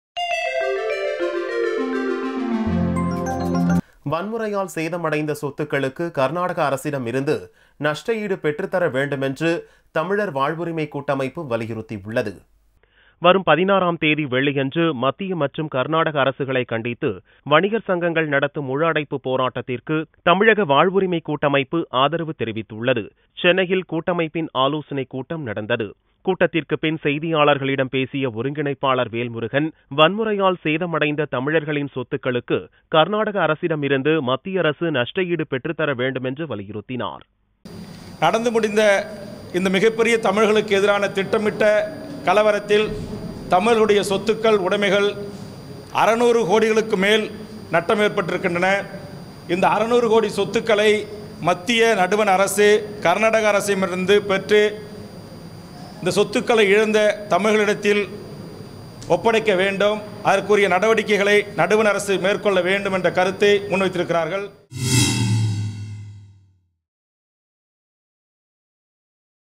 Karnataka Govt.., As give Compensations to TN : T. Velmurugan
Tamizhaga Vazhvurimai Katchi Leader T.Velmurugan urges Karnataka Govt.. as give compensations to TN - Dinamalar sep 15th 2016 For more videos